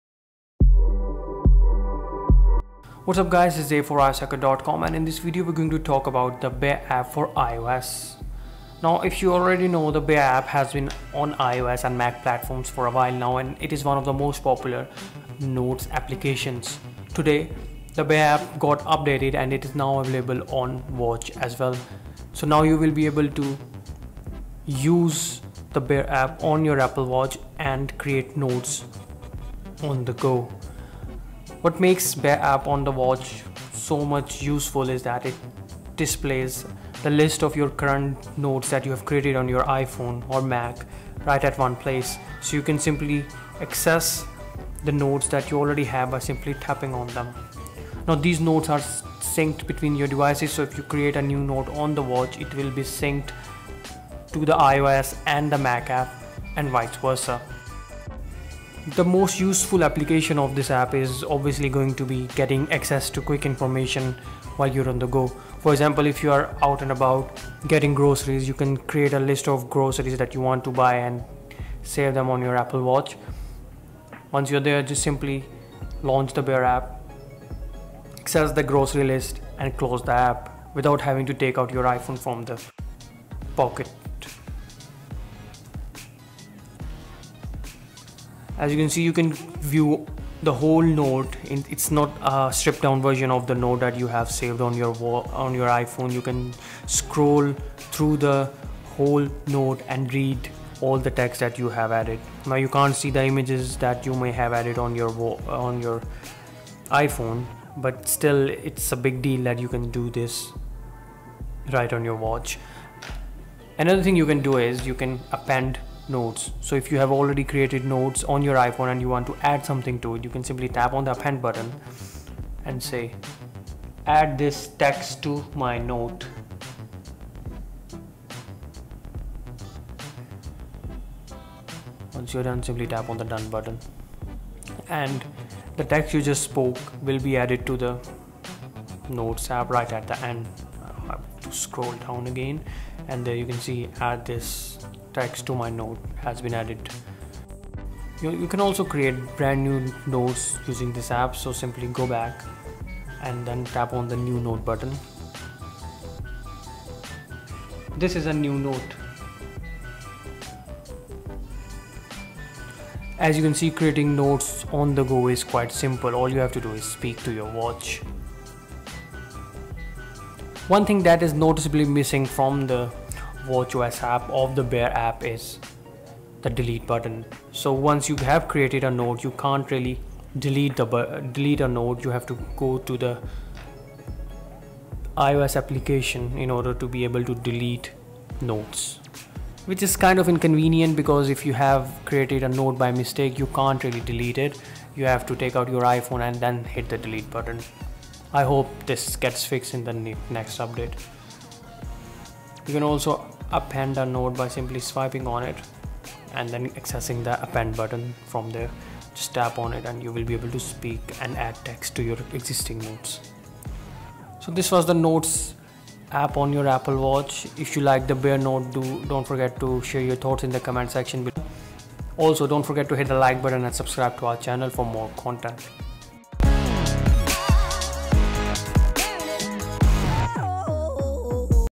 Bear App Comes To Apple Watch
The popular Note-taking app for iPhone, iPad and Mac has finally made its way to the Apple Watch. Now with the updated app you can make read your Bear notes, create new notes and make amendments to existing notes.